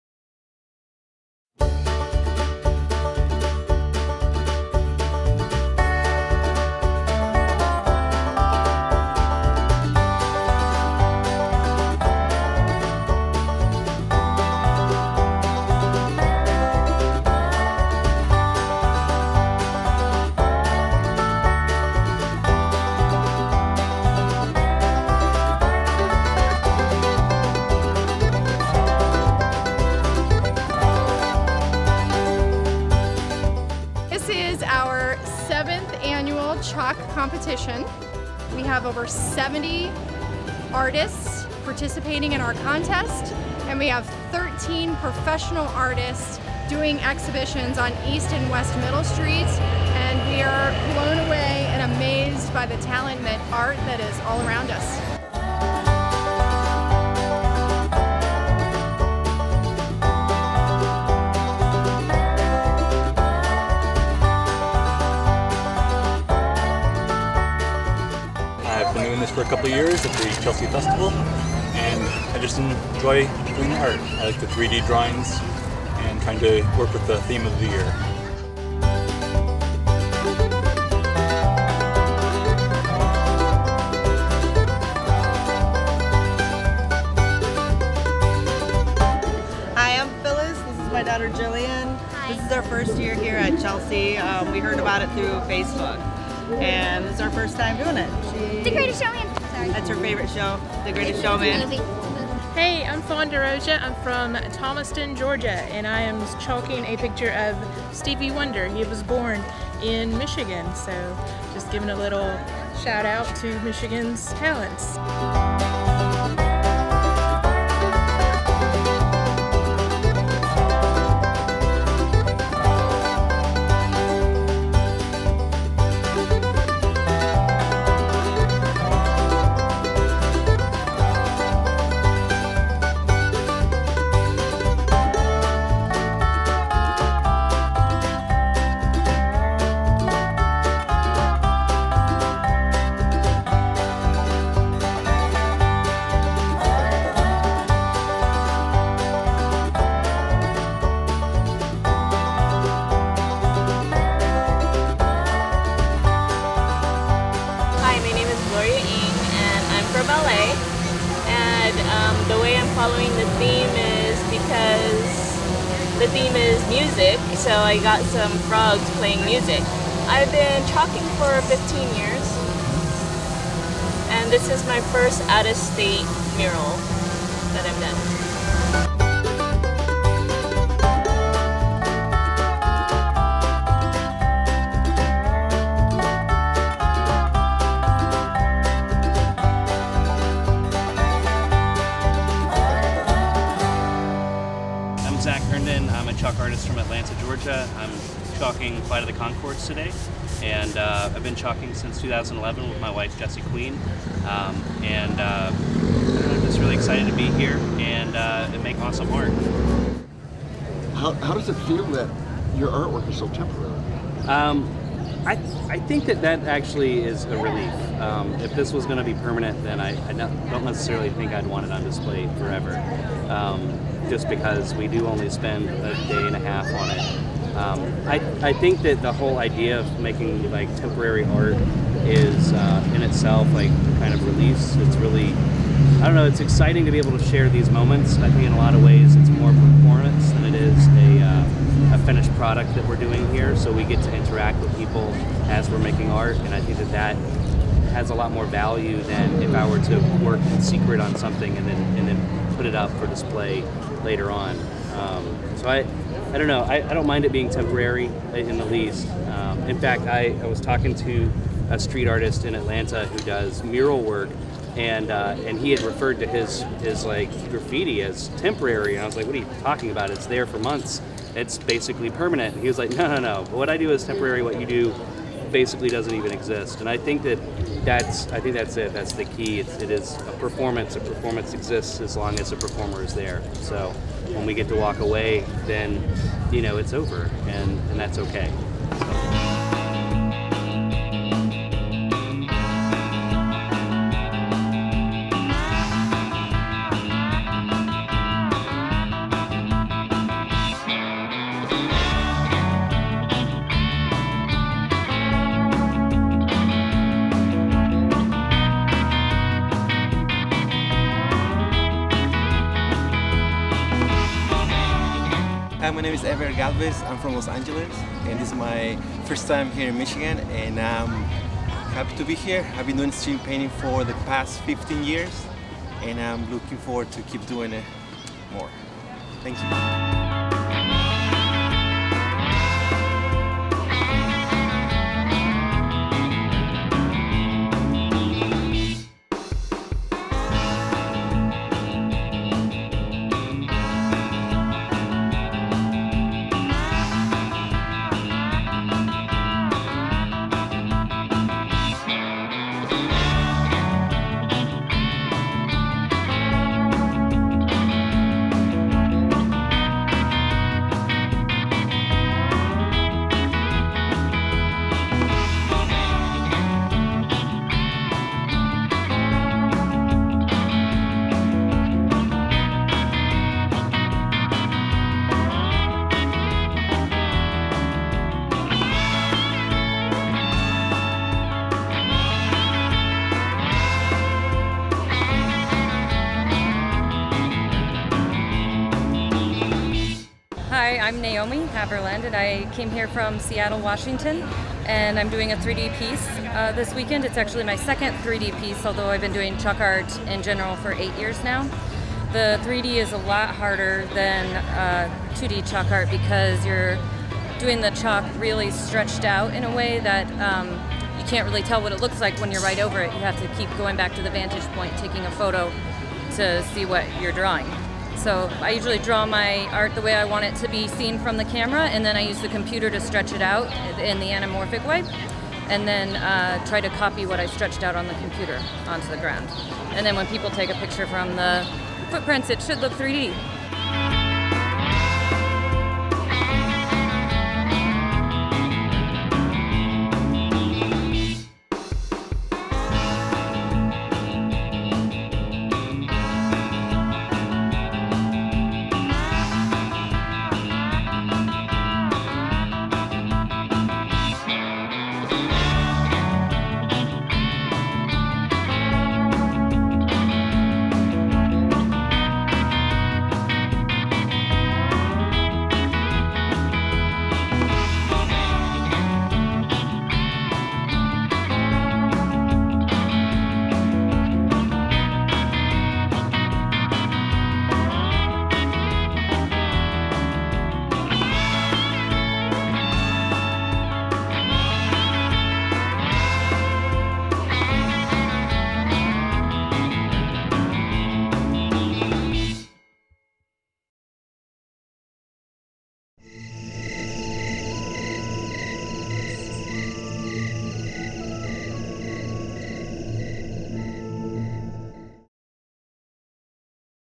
Chalk Art2018
Chelsea, Michigan's Sounds & Sights Festival Chalk Art featuring professional chalk artists from around the country, presented by the Chelsea Area Festivals & Events. Video by Randall Lee, RK studios, LLC, Chelsea, MI.